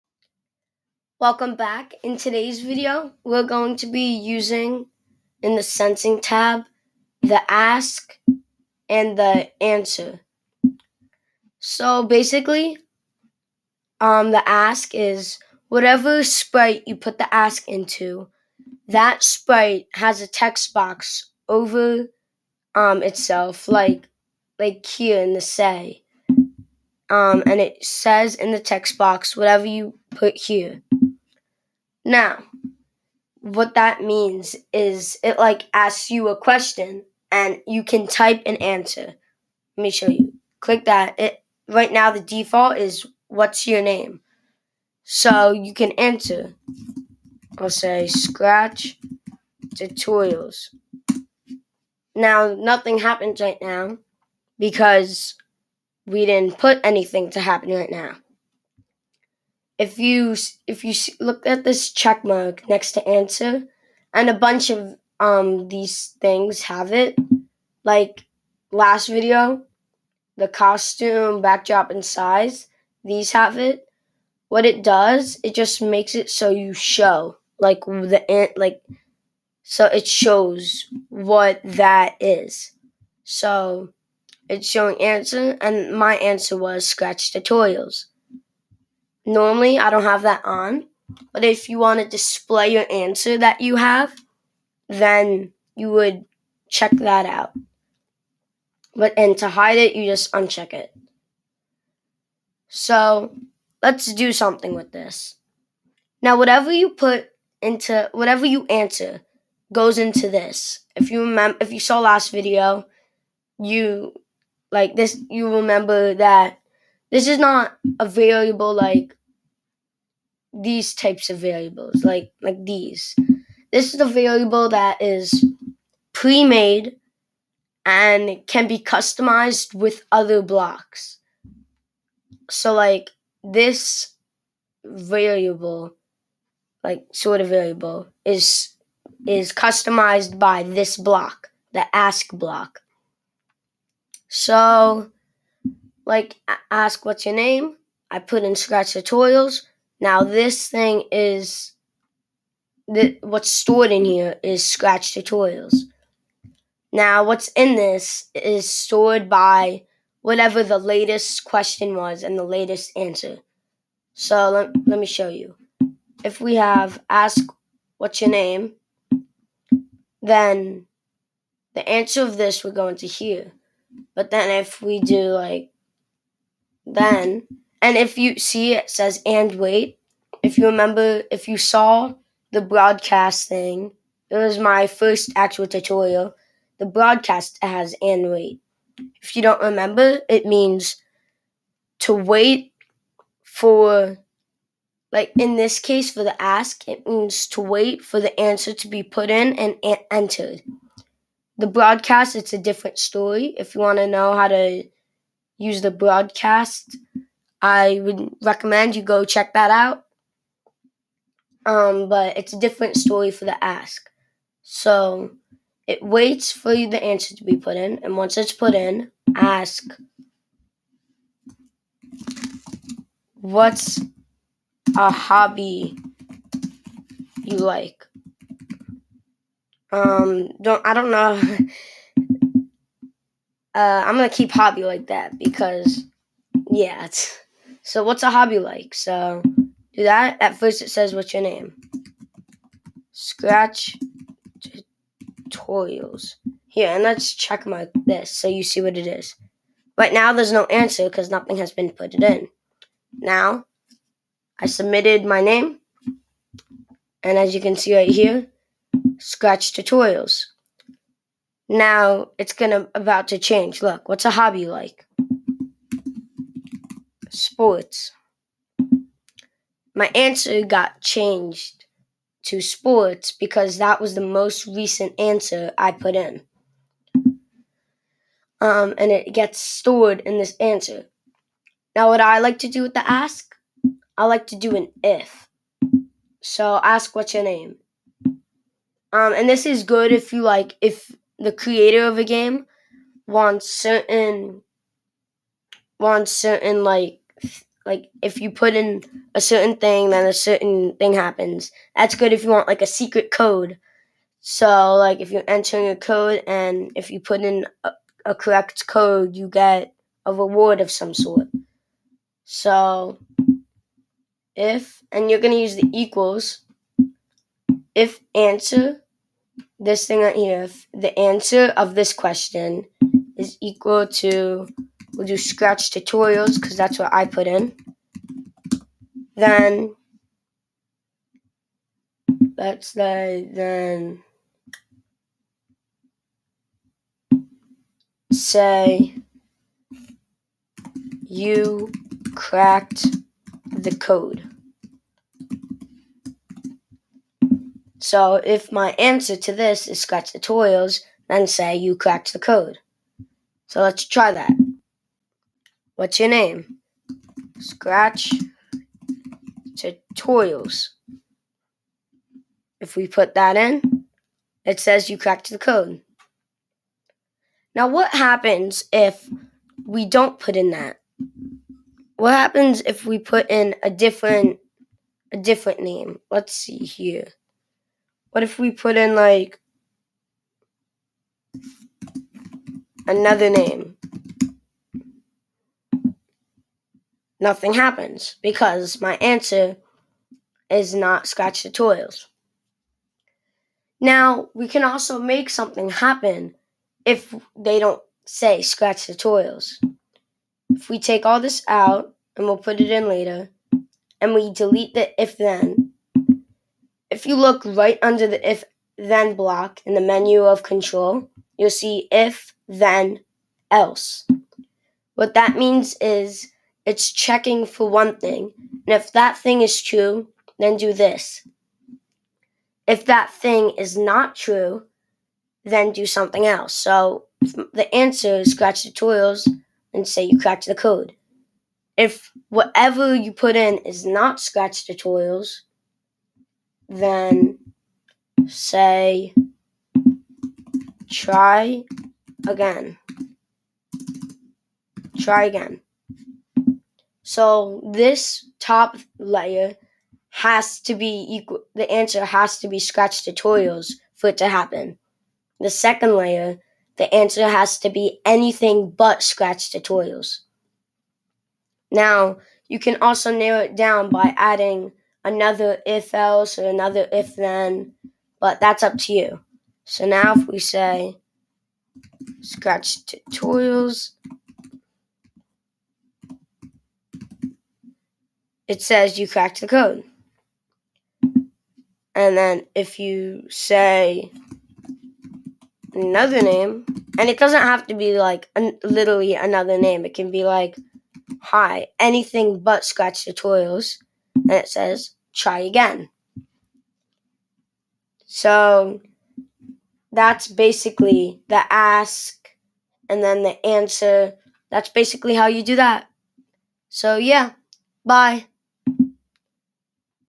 How to Use the Ask and Answer Blocks in Scratch
In this video, I show how to make a sprite ask a question and make the player answer it.